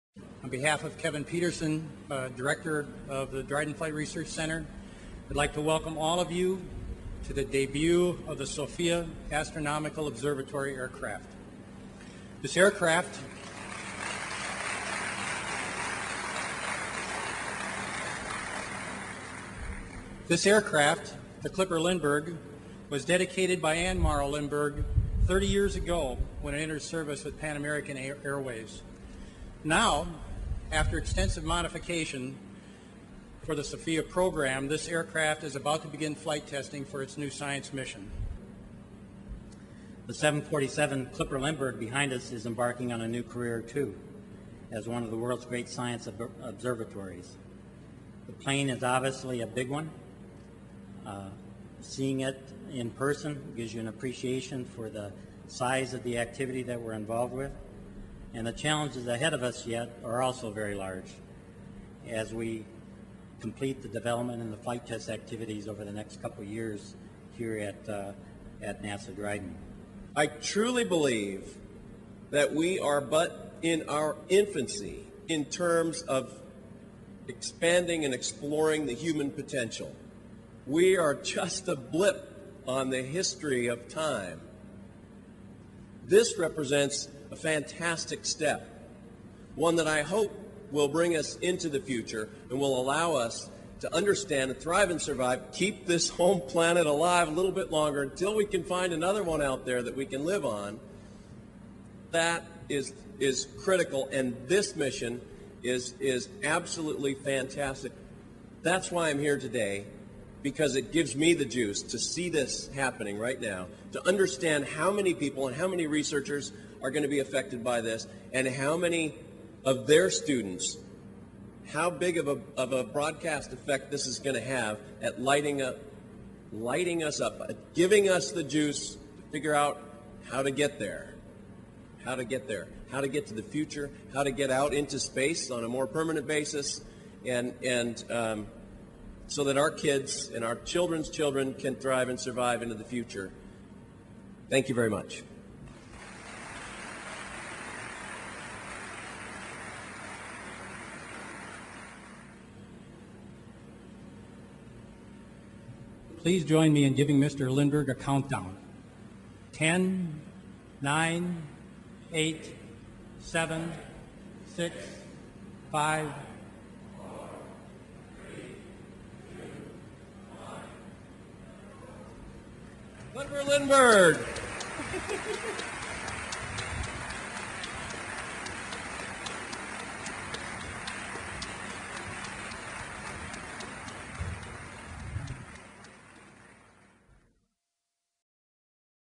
Debut of NASA's SOFIA at Dryden
This 3 minute 10 second video clip shows the relocation flight of Stratospheric Observatory for Infrared Astronomy from Waco, Texas to Dryden Flight Research Center.